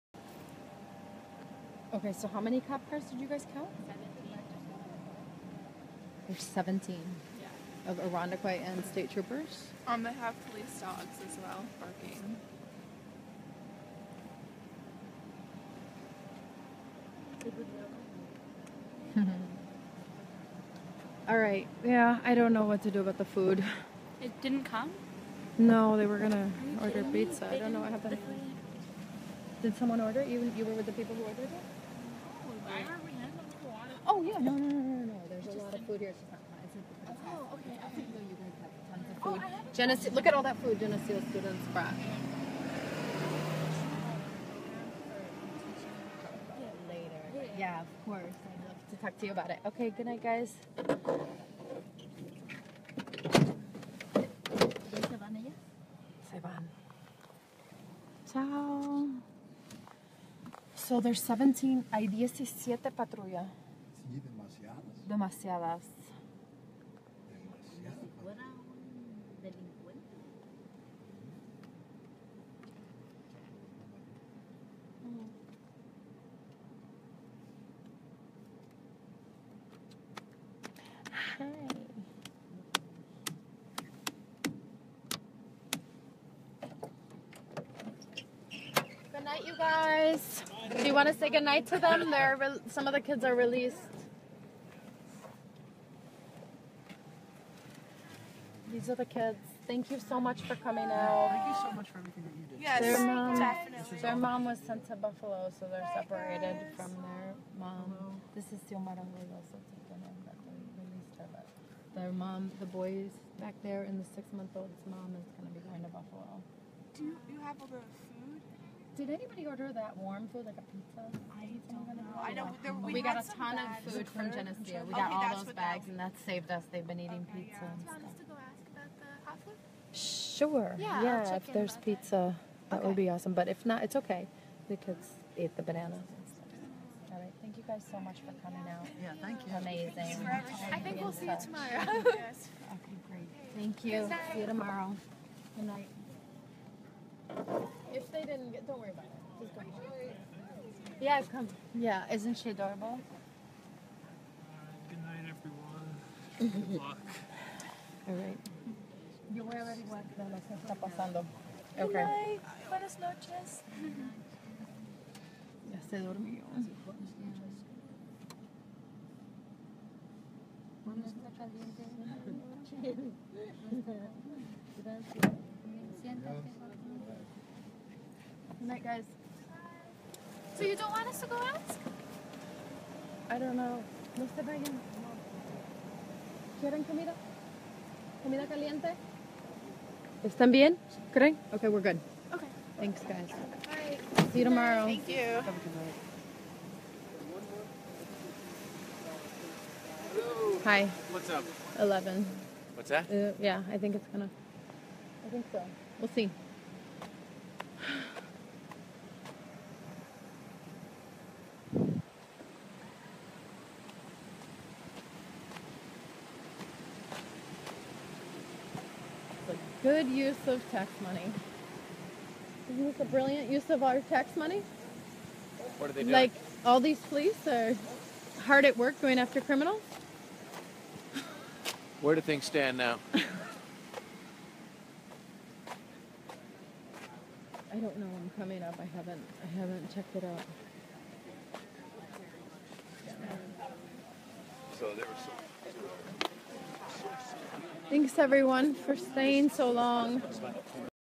Family taken by border patrol, video from their van, part 2
Video from Carly Fox, of the Worker Justice Center of New York, documenting Geneseo, NY police stopping two mothers and their five children and then calling the border patrol to detain and hold them. Geneseo students and residents gathered to bear witness. This happened on the night of March 23, 2017. The demonstration moved from Geneseo to Rochester, NY, where the border patrol station is located on Pattonwood Drive in Irondequoit.